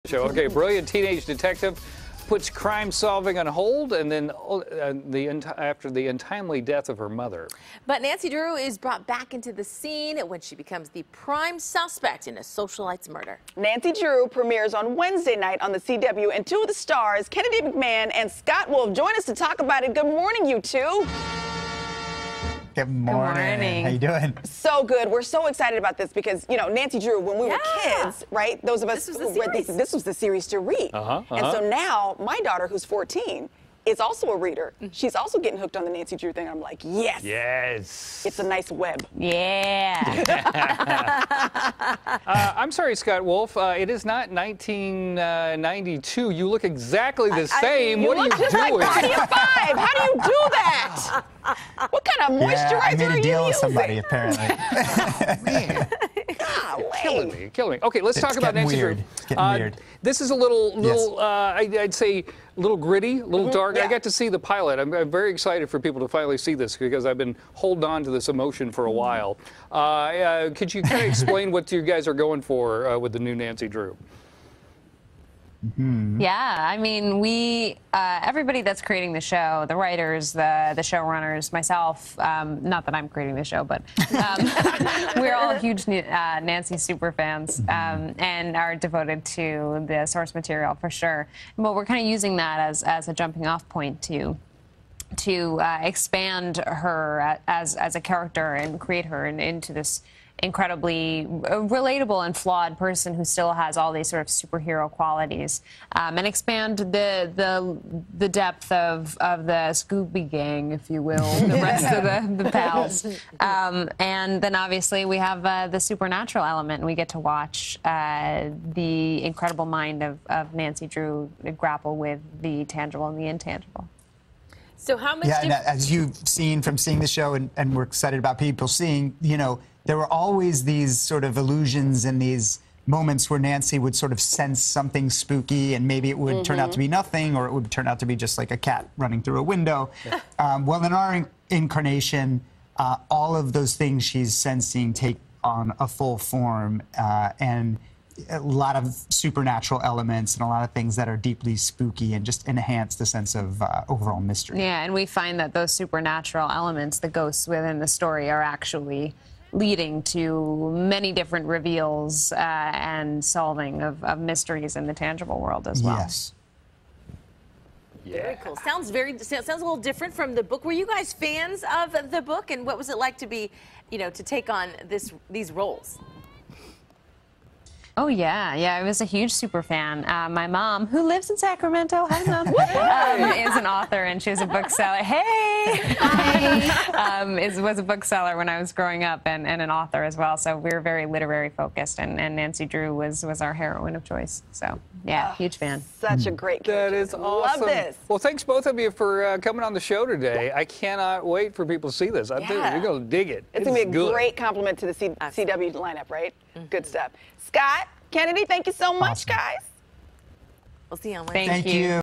New Series “Nancy Drew” Cast
The Hosts are chatting with Kennedy McMann and Scott Wolf about the new show Nancy Drew which premieres Wednesday, October 9th at (9:00-10:00 PM ET/PT) on the CW!